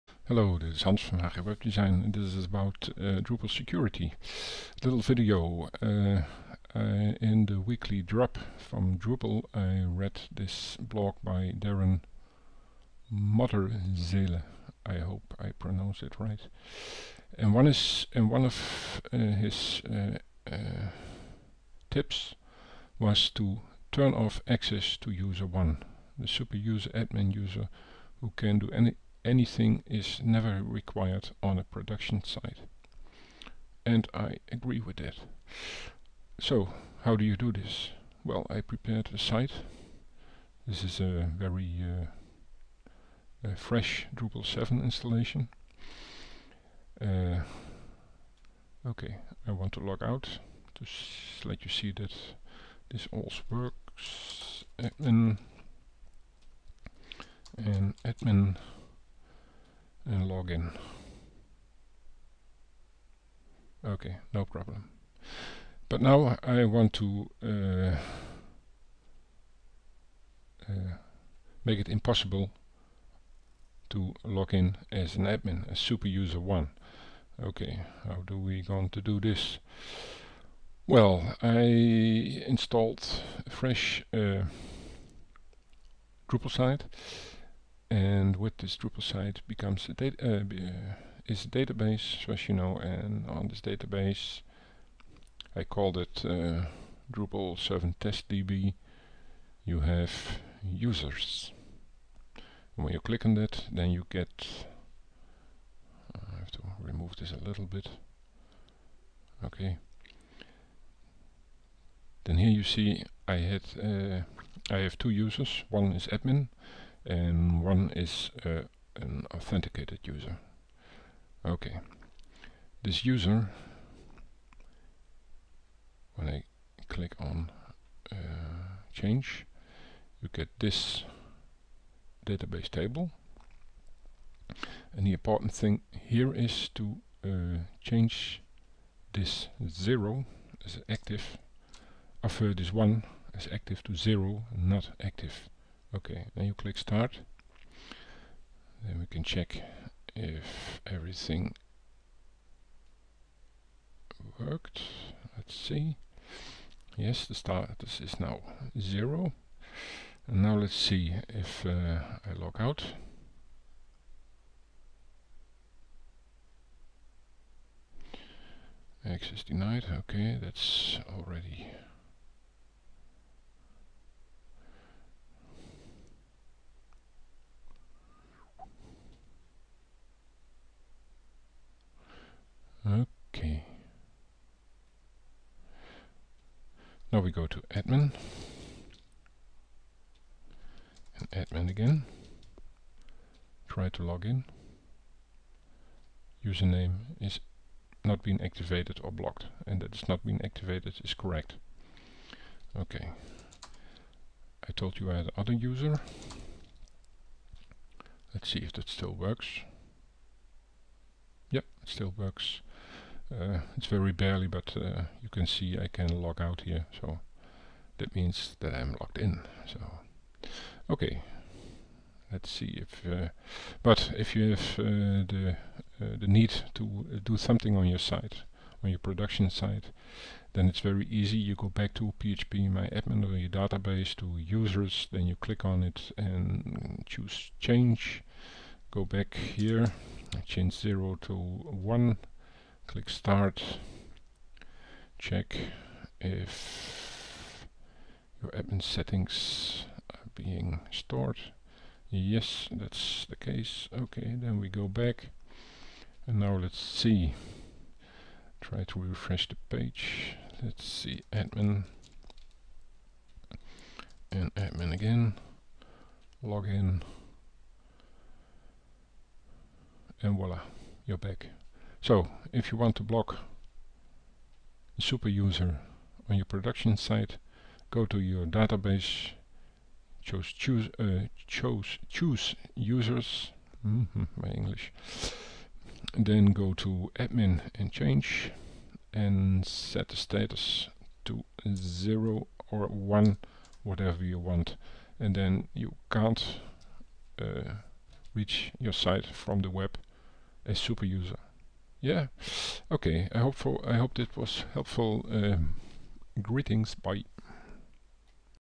drupal 7 block access user one
Turn off access for user 1. The super admin user who can do anything is never required on a production site. Turn it off by setting status=0 in the database.